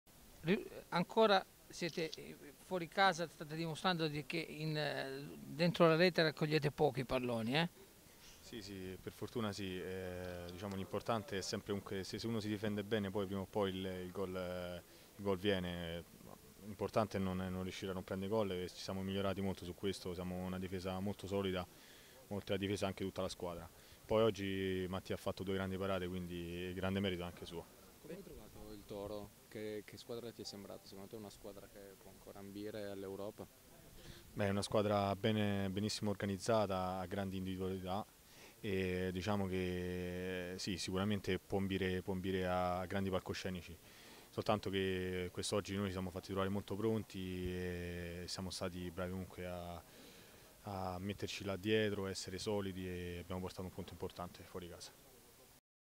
Biraschi:”Portiamo a casa un punto importante”
SPORT(Torino)."L'importante è non prendere gol, siamo una difesa molto solida e Mattia ha fatto due splendide parate. Il Toro ha grandi individualità e ben organizzata, può ambire a grandi palcoscenici, ma oggi ci siamo fatti trovare pronti e abbiamo portato a casa un punto importante." Intervistato in zona mista, Davide Biraschi commenta così lo 0-0 contro il Toro.(Alessandra Cataleta/alaNEWS)

♦ Alanews crea e produce contenuti giornalistici multimediali per le aziende editoriali, fortemente orientati ai nuovi media.